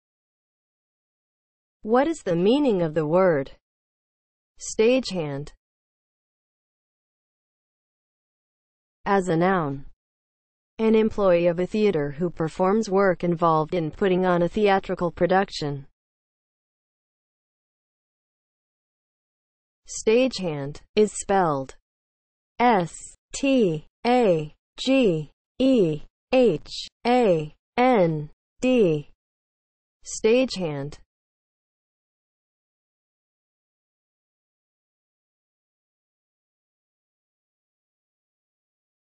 What is the meaning of the word STAGEHAND?
Definition and spelling of the word STAGEHAND.

(00:00) Intro
(00:08) As a noun
(00:18) Spelling

As a noun (00:08):
• an employee of a theater who performs work involved in putting on a theatrical production